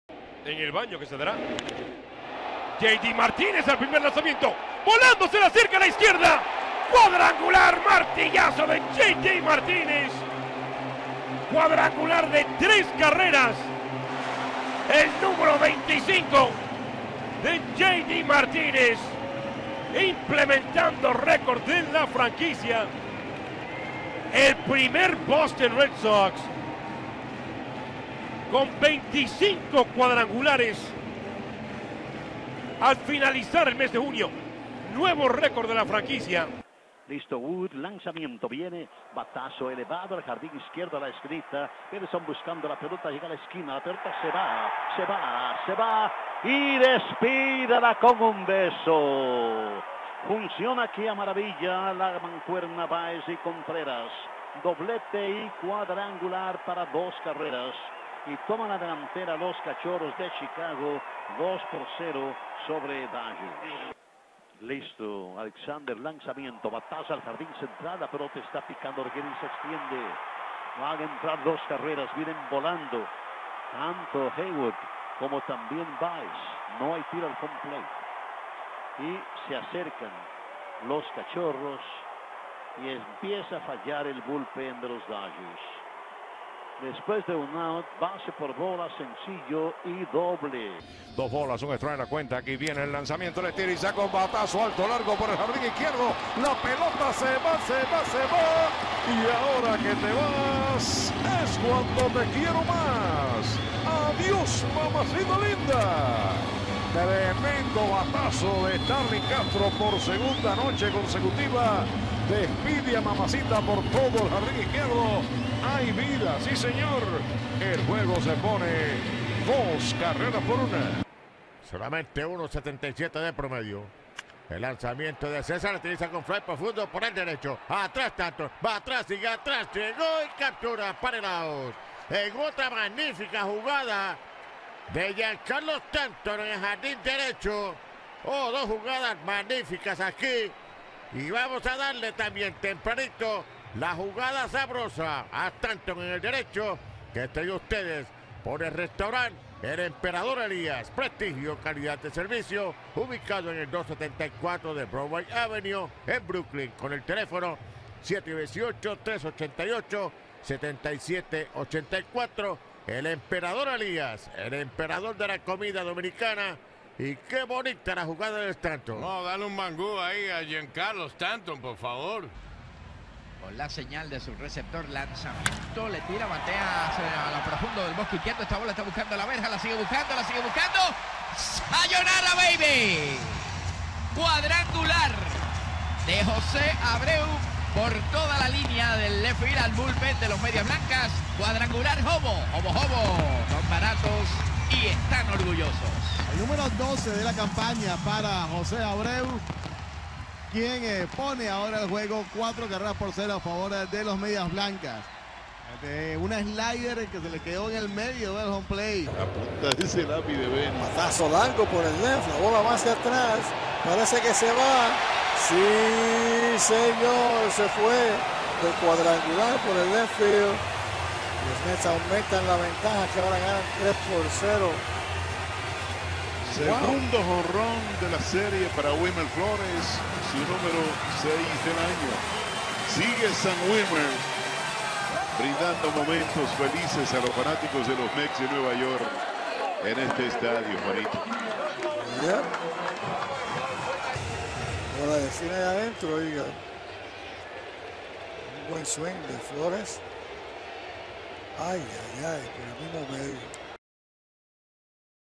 Lo mejor del miercoles en Grandes Ligas
El 25to HR de J.D., las cuatro remolcadas de Contreras y la atrapada de Stanton lideran la jornada